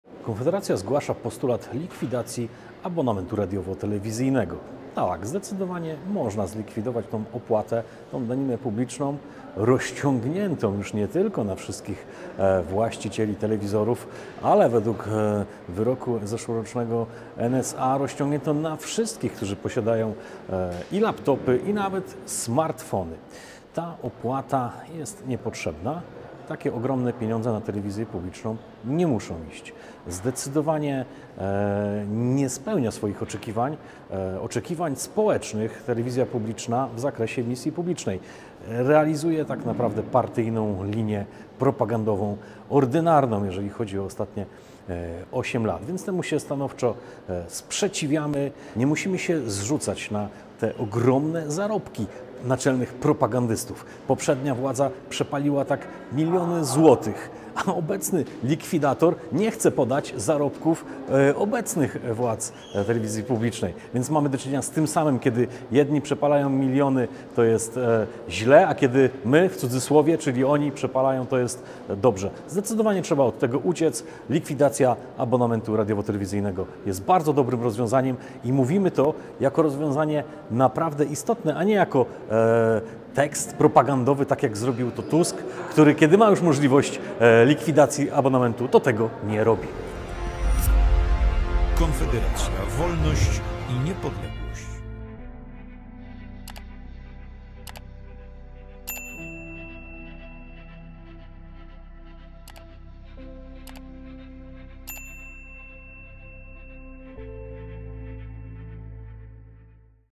Zlikwidować abonament radiowo-telewizyjny! Krzysztof Tuduj
Krzystzof Tuduj, poseł Konfederacji

Zapraszamy na kanały naszych liderów: @krzysztoftuduj5136

Polub 👍
Skomentuj 💬
Podaj dalej ➥